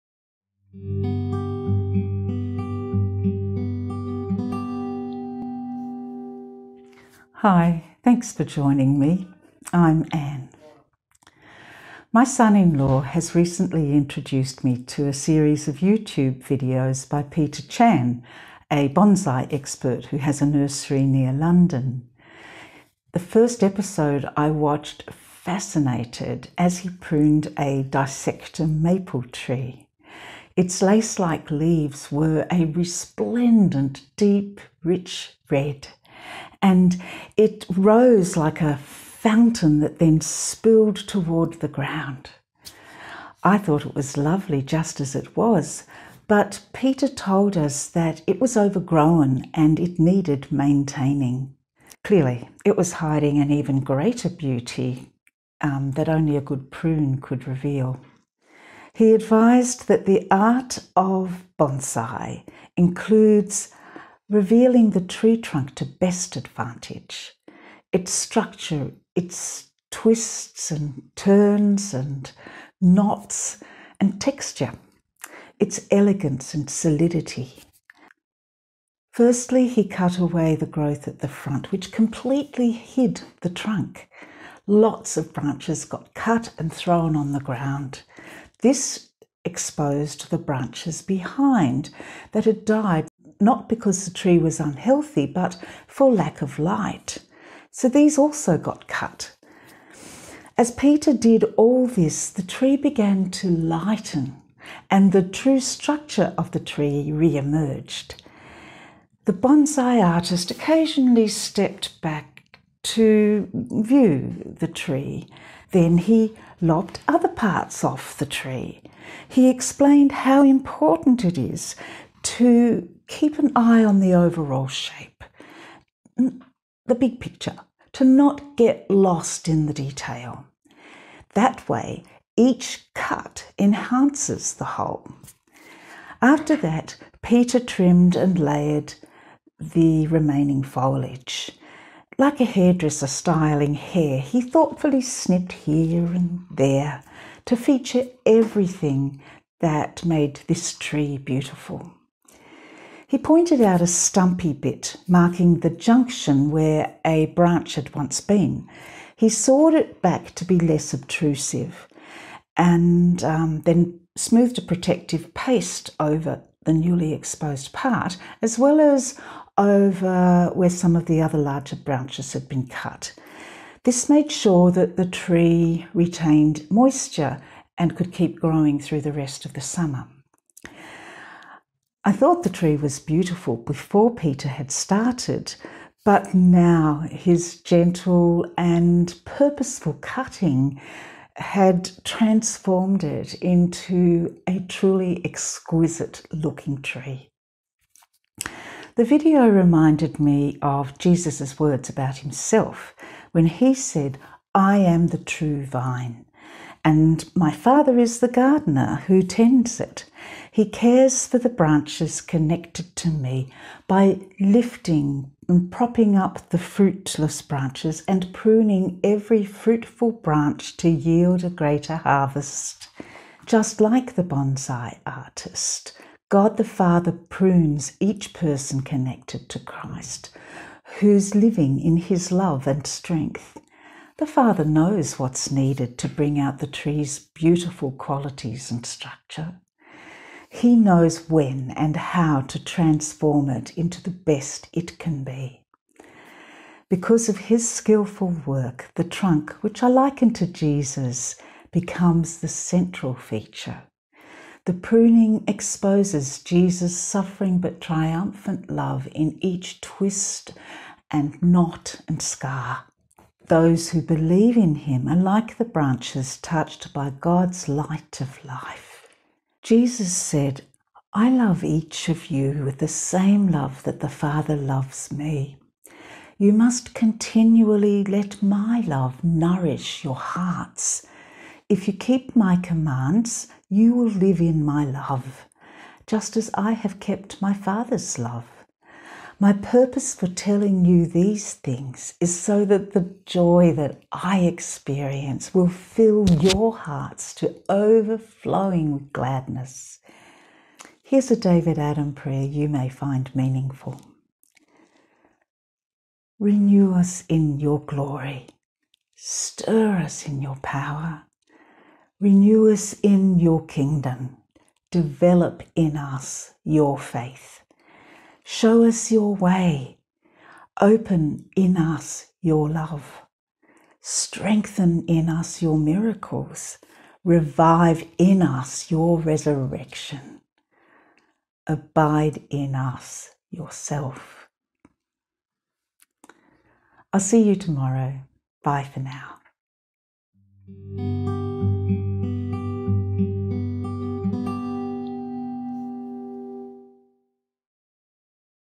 Evenings with Ann S2 E16 – Lesson of the Bonsai Tree
A bonsai artist can transform a beautiful tree into one that is exquisite. Jesus tells us that he is the true vine, and his Father is the Gardener who tends the vine so it yields more fruit. Like a bonsai artist, the Father prunes and shapes each person connected to Christ so that Jesus becomes the central feature. This reveals his suffering-but-triumphant love and exposes each believer to God’s light of life.

Bible Readings:
John 15:1-2 (NIV/TPT); John 15:9-11 (TPT)

Prayer: Revelations, by David Adam
Reveal us in your glory
Stir in us your power
Renew in us your kingdom
Develop in us your faith
Show us your way
Open in us your love
Strengthen in us your miracles
Revive in us your resurrection
Abide in us yourself

Reference:
David Adam, 1995, Tides and Seasons: Modern prayers in the Celtic tradition.